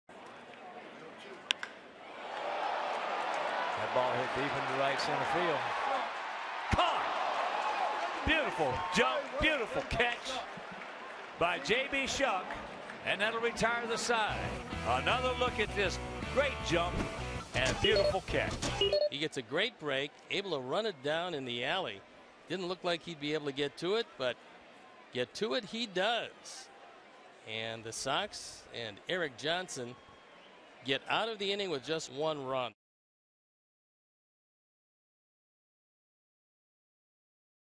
CWS@NYY: Shuck runs 99 feet to make the catch
9/27/15: Statcast tracks the distance covered and route efficiency for J.B. Shuck's run-saving catch in the bottom of the 1st